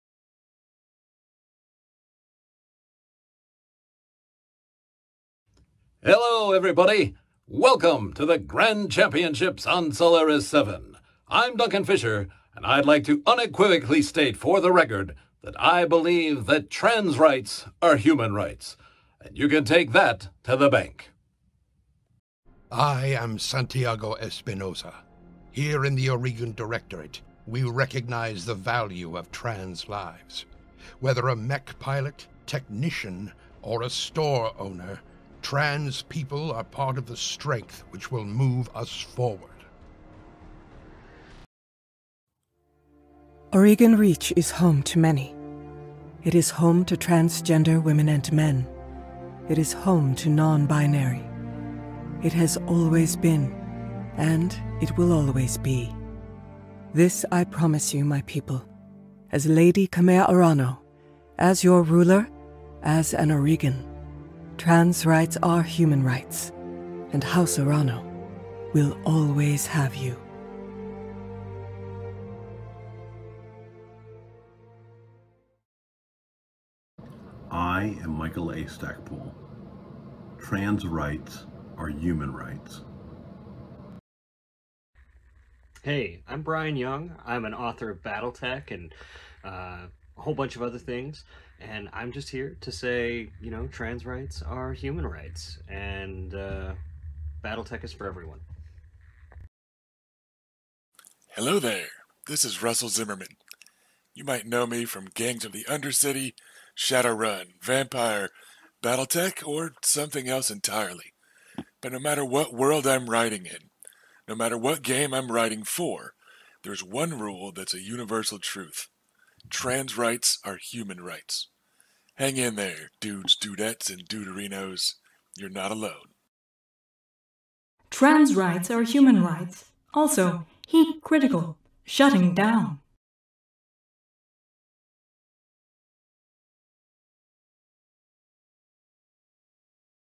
BattleTech - Support For Trans Rights From Those Who Helped Shape It
As a personal initiative to support the trans community within the BattleTech fandom I reached out to some of the incredible people who make BattleTech what it is. From the writers who create some of our favourite stories, to the voice actors who brought characters we love to life.

Thank you to these incredible voices that rose up in support, who were not afraid to speak up on behalf of some of the most vulnerable in our society. Because trans rights, are human rights.

▶ George Ledoux - Voice Actor, as Duncan Fisher in MechWarrior 4: Mercenaries
▶ Trendane Sparks - Voice Actor, as Santiago Espinosa in HBS BattleTech
▶ Gin Hammond - Voice Actor, as Kamea Arano in HBS BattleTech
▶ Michael A Stackpole - BattleTech Author
▶ Bryan Young - BattleTech Author
▶ Russell Zimmerman - BattleTech Author
▶ Bailey Wolfe - Voice Actor, as Bitchin' Betty, MechWarrior 5: Mercenaries

Trans pride chevron by Detocroix

BattleTech is for Everyone